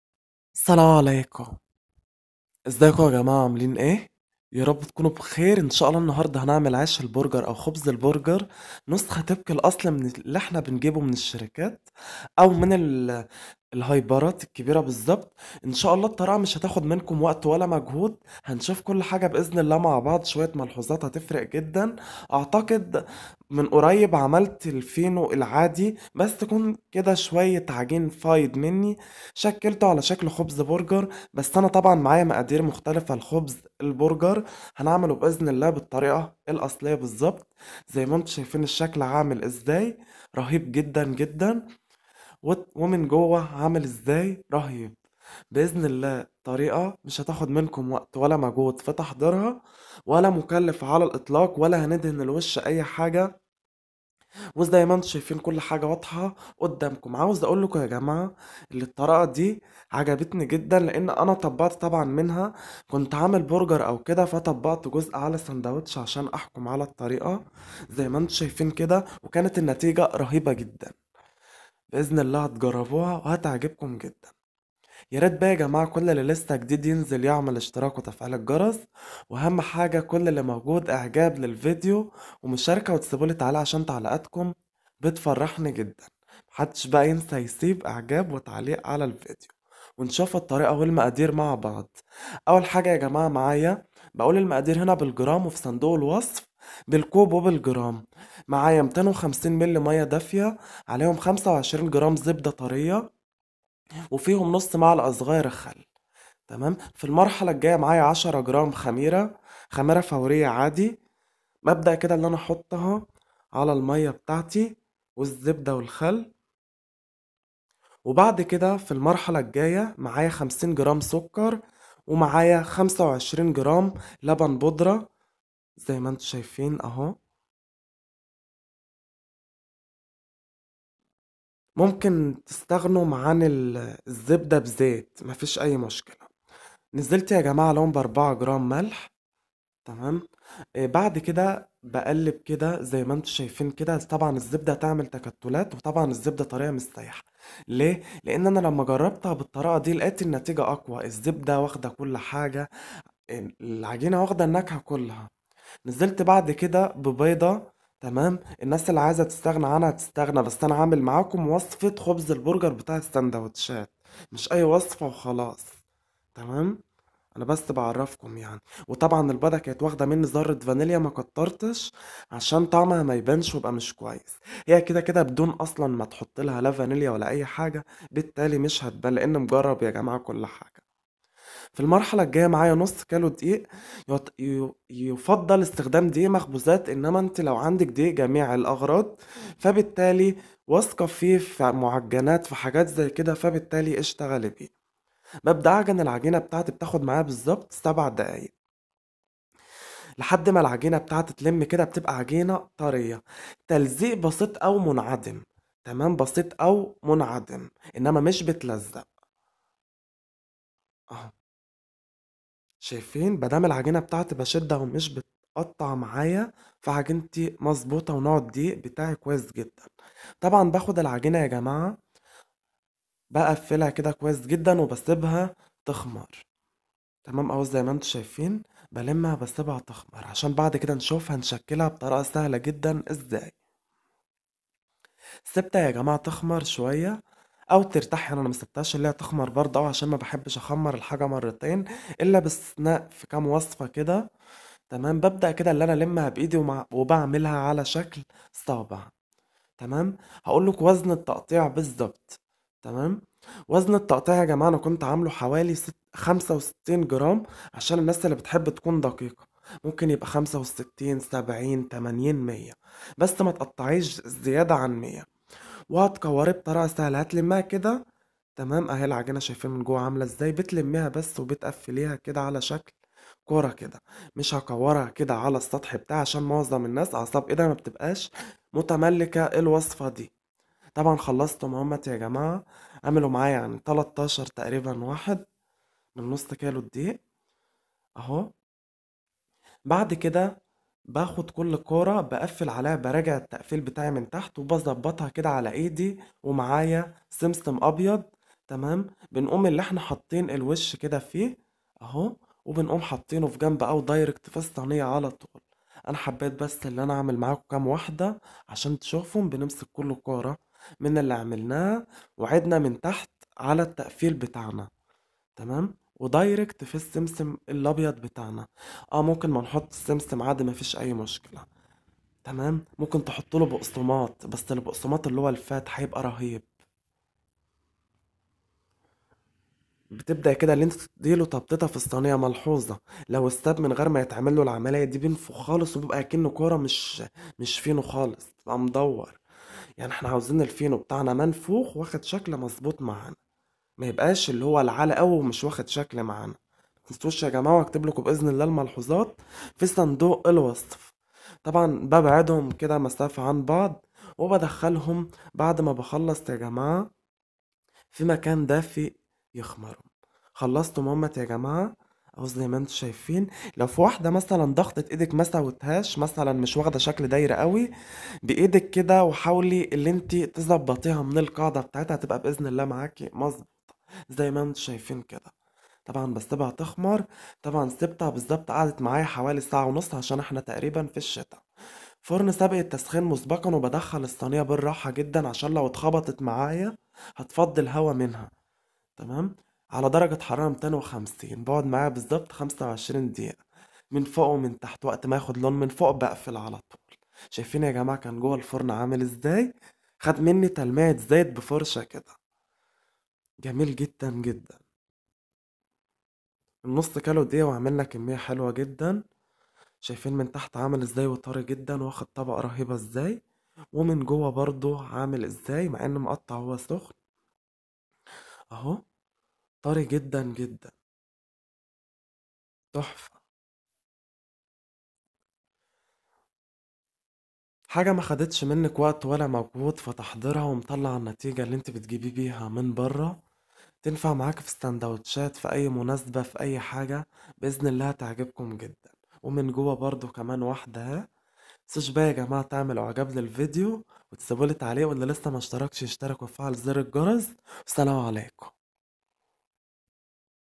انسوا اي خبز برجر وجربوا خبز برجر هش قطني طري منفوخ جدا ناعم بمكونات في كل بيت !!
شكرا لك علي المشاهدة , متنساش تعمل اشتراك في القناة وتفعل زر الجرس🔔 عشان يوصلك كل فيديو جديد .

المقادير
500جرام دقيق مخبوزات 4كوب أو استخدام دقيق واثقين فيه في المخبوزات بتاعتكوا
50جرام سكر ناعم مش بودرة ربع كوب
بيضة +ذرة فانيليا
نقطتين خل
25جرام زبدة طرية مش سائحة ولاساقعة يعني معلقة كبيرة فقط
4جرام ملح نص معلقة صغيرة
250ملي ماء دافئ كوب تقريبا
للوجه
(سمسم ابيض )
الملاحظات
نسيب الزبدة زي ماشوفتوا كدة كدة هتخلط بالعجين هتفرق جدا في الطعم
العجن الكويس لحد مالعجينة تكون طرية وتلزيق شبه منعدم
الضغط علي العجين بعد التشكيل أمر ضروري
الترك لمدة ساعة ونص لتخمر مرة أخري حتي لا يصبح الخبز مكتوم
نسبة السكر اللي في العجينة مظبوطة تغنيكي انك تحطي اي حاجة لأن فيه قاعدة بتقول في حالة وجود سكر وجود لون
عدم خبط الصينية جامدة أثناء دخولها الفرن حتي لا تفقد الهواء الناتج عن شغل الخميرة
عدم دهن الصينية زيت
الخبز في الرف الاول علي درجة حرارة 250 فوق وتحت وقت ماتاخد لون من الاعلي تطفي النار من الاعلي مدة التسوية 25دقيقة
تلميع الوجه بعد التسوية بزيت

وزن قطعة العجين 65جرام ممكن 70جرام معاكي لحد 100جرام
الناس اللي معندهاش ميزان
تشوف الليمون وتشكل علي مقاسه